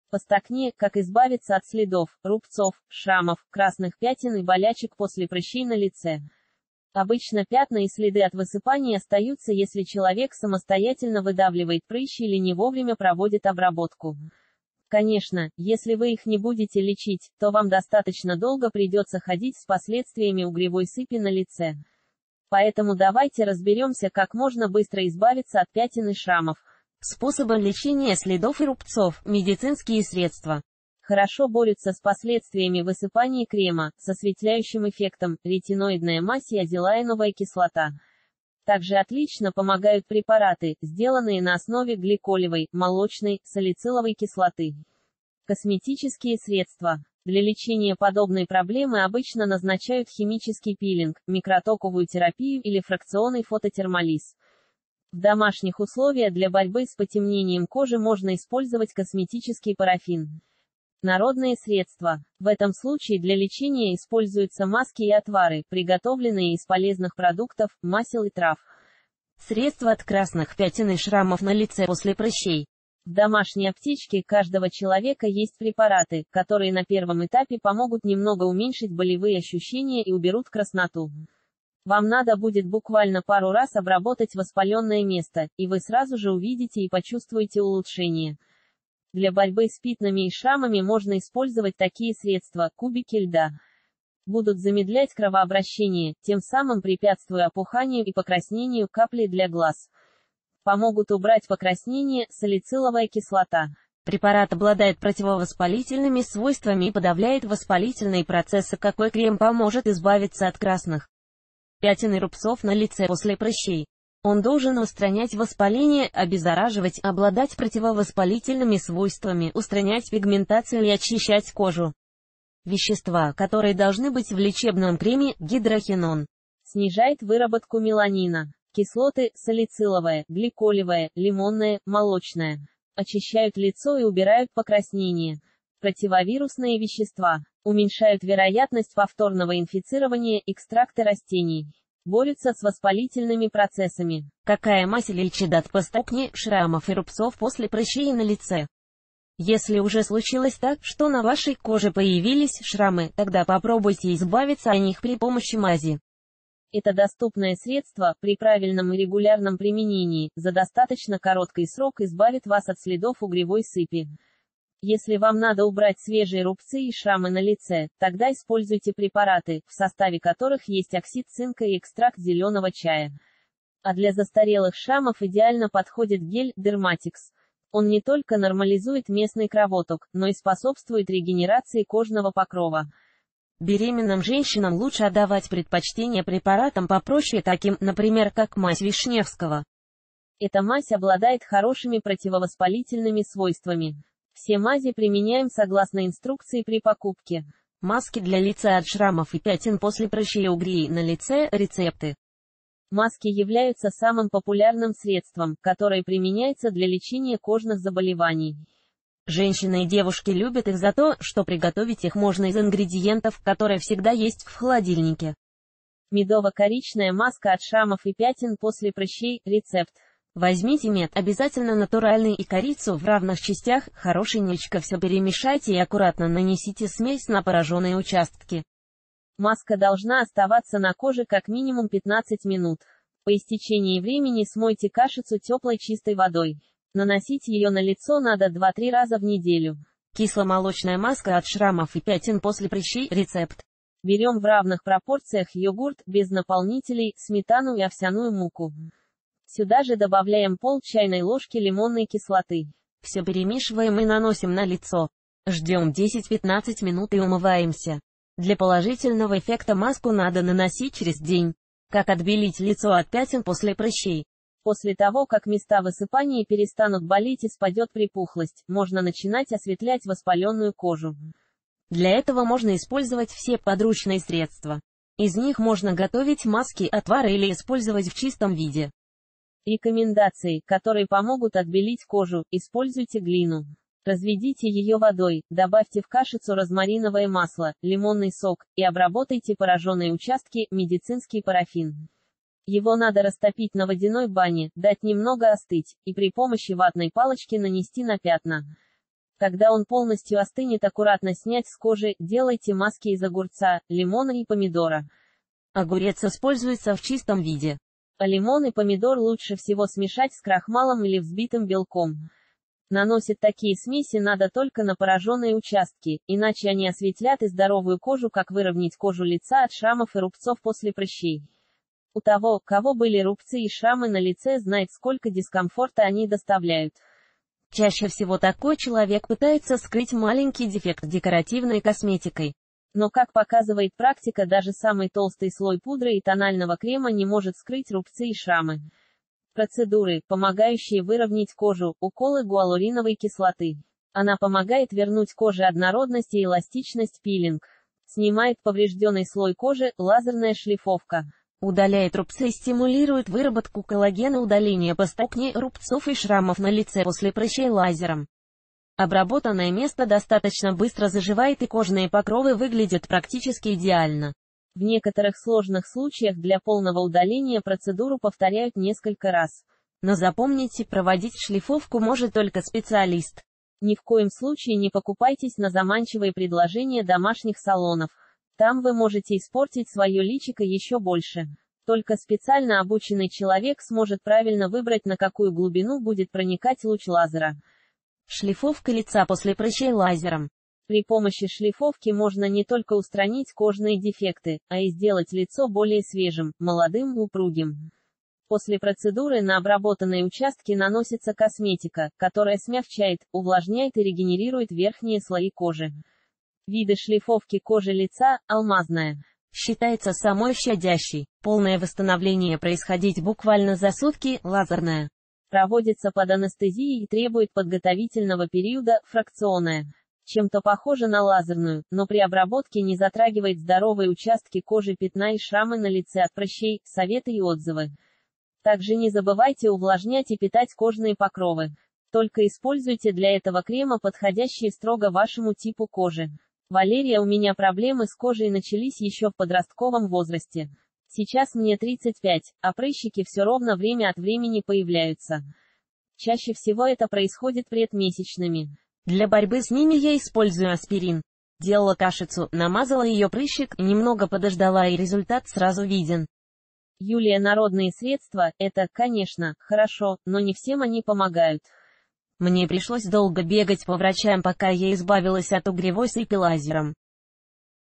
Постакне — красные пятна от угрей, рубцы и шрамы: как избавиться? Как восстановить лицо при постак.
Постакне — красные пятна от угрей, рубцы и шрамы: как избавиться? Как восстановить лицо при постакне после прыщей кремом, мазью, масками для лица от шрамов и пятен после прыщей и угрей: рецепты